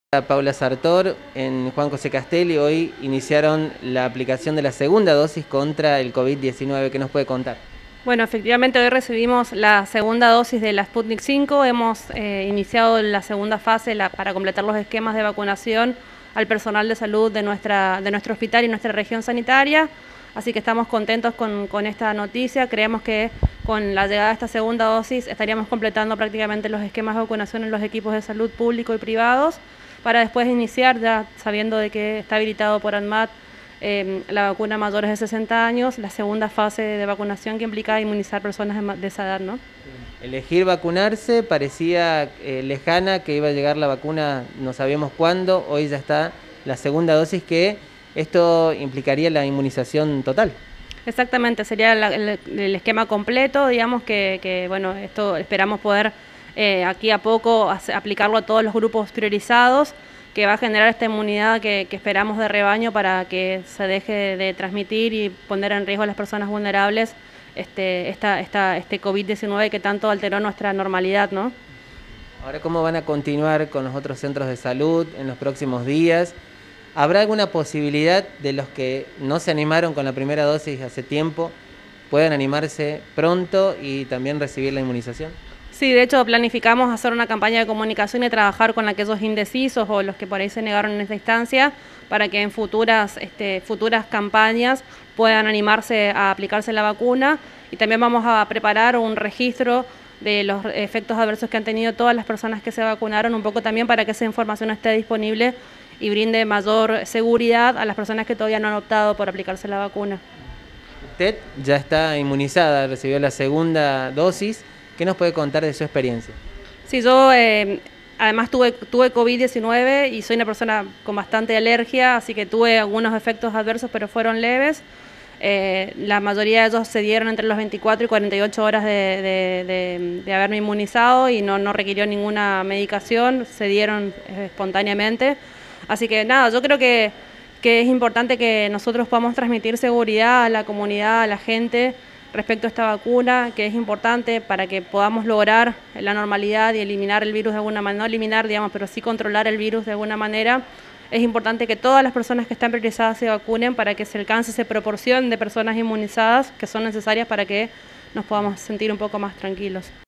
N9 - PLAN DE VACUNACIÓN: ETAPA II - CHACO
INOCULARON AL PERSONAL SANITARIO DE RESISTENCIA, SÁENZ PEÑA Y CASTELLI
APLICAN LA SEGUNDA DOSIS DE SPUTNIK V A PERSONAL SANITARIO
ASÍ FUE EL OPERATIVO EN JUAN JOSÉ CASTELLI